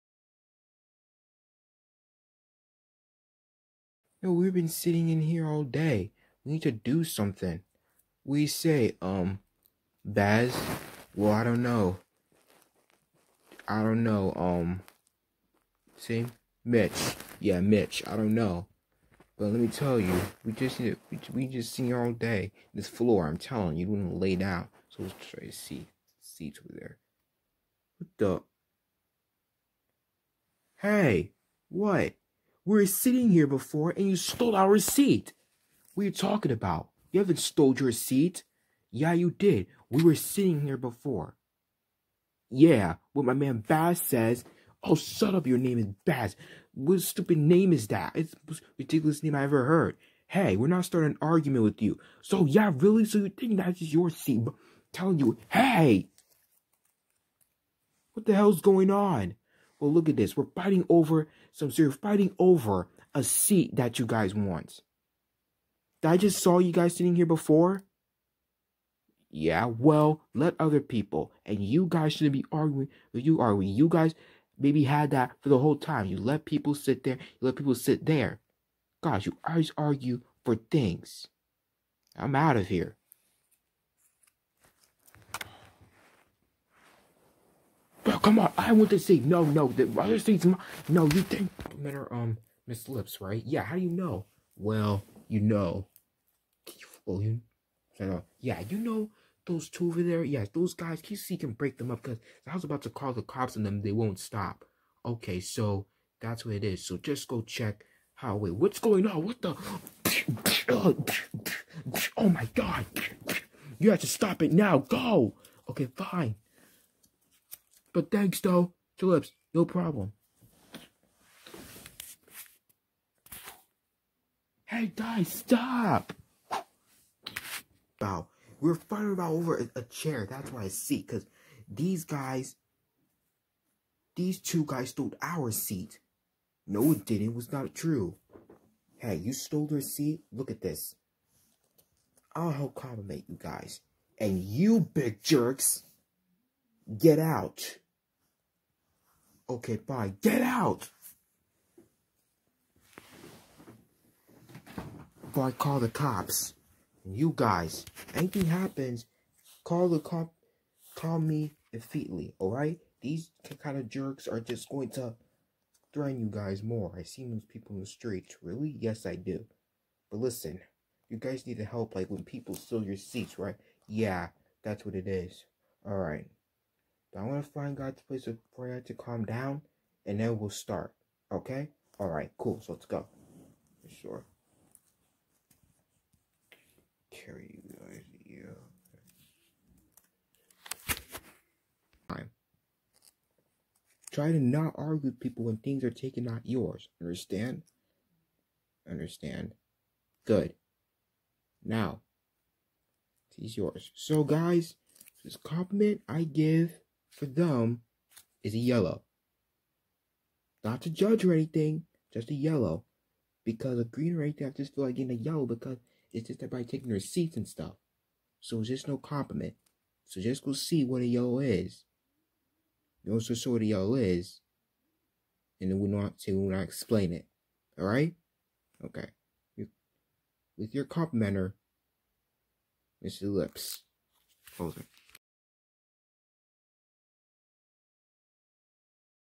Episode 6: Argument to deal with
Two guys Baaz and Mich fight over by their seat taken.